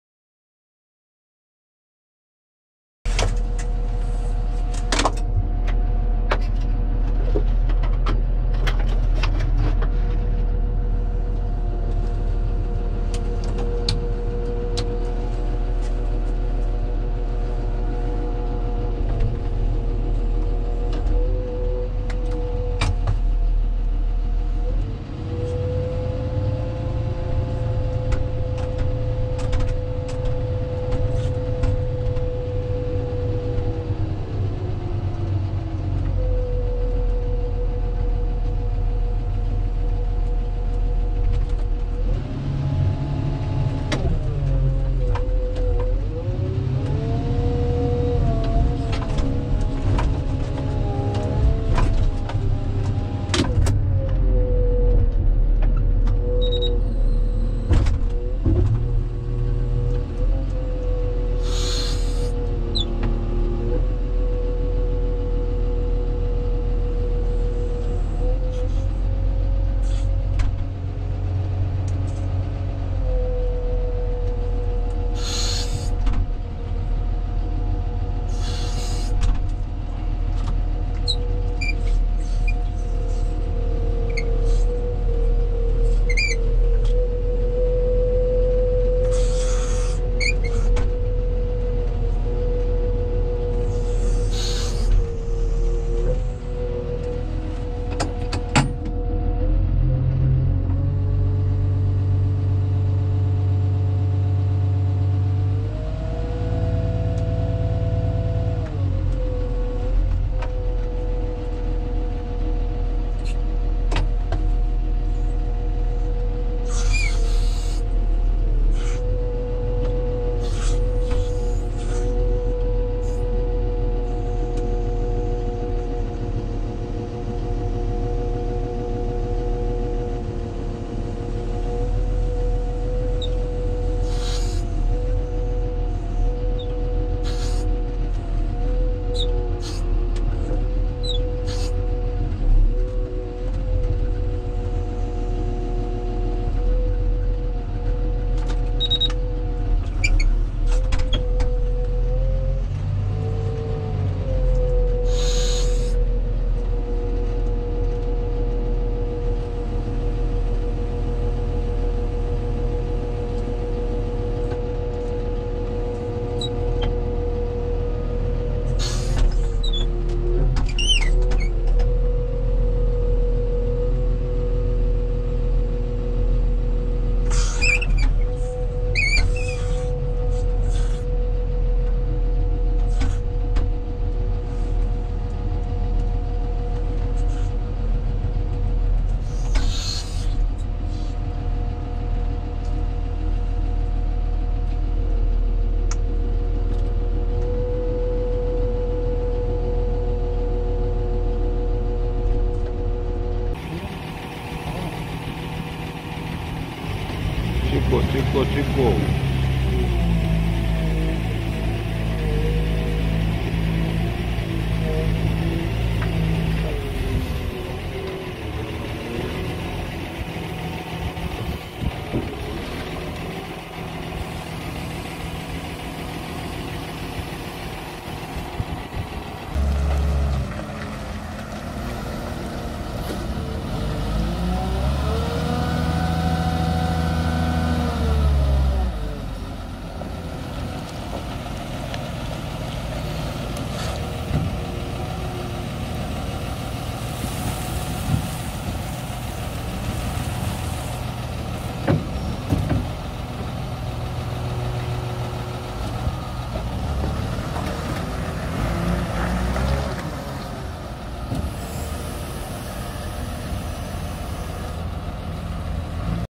로더운전기능사 실기시험 준비 꿀! 실내 촬영 영상(화성신진중장비전문학원) 100%합격 가이드
건설중장비 로더 - Heavy Construction Equipment Loader
오늘은 로더운전기능사 꿀팁! 두번째영상으로 실내 촬영영상입니다.
외부에서 촬영만 보신분들은 내부에서 로더운전기사가 어떻게 장비를 조작하고 운영하며 실재 실내에서 운전시 간접경험을 통한 현장감을 배가하고자 만든 영상입니다. (영상은 참고하시고 실기는 당해 신진학원에서 받으신다면 100%합격하실거라 생각됩니다.)
본 촬영을 위해 도움을주신 [화성신진중장비학원]관계자 분들의 협조 및 지원에 감사드립니다.